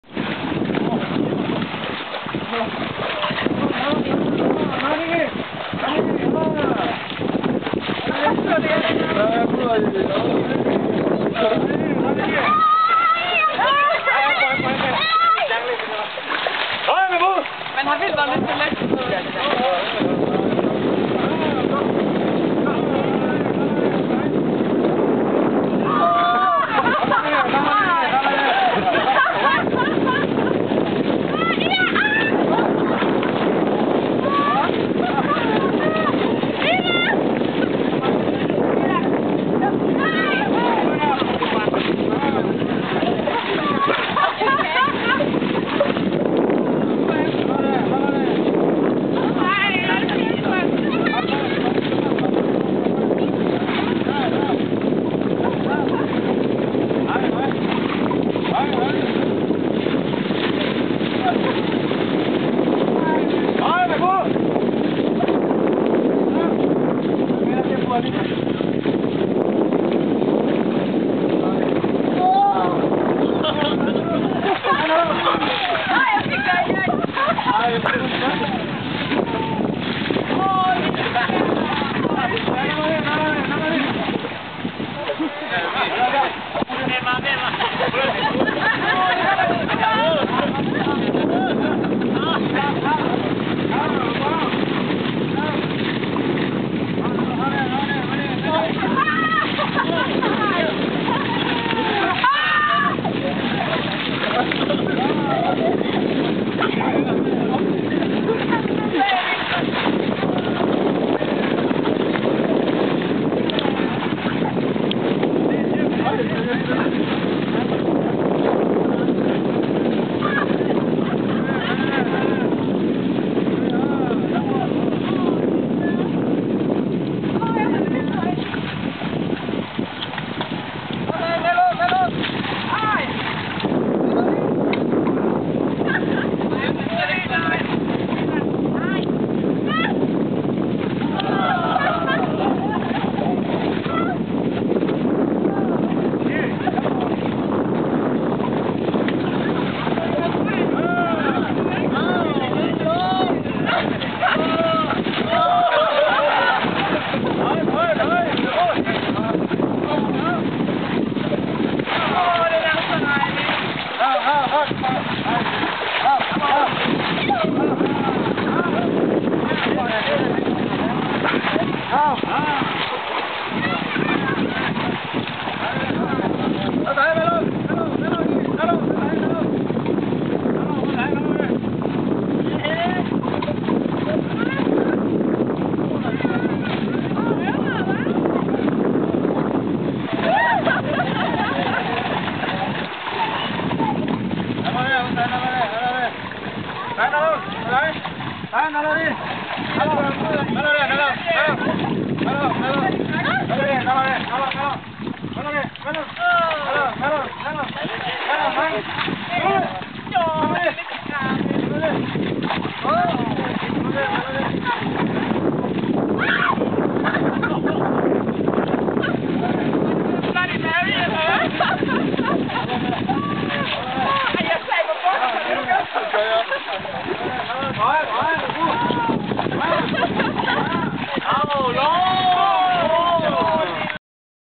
Mud Fight!
While the idea was to give the elephants at the Thai Elephant Home Nursery a mud spa, the guests decided they could do that AND have a mud fight! I'm not sure who loved the mud more. the elephants or the guests!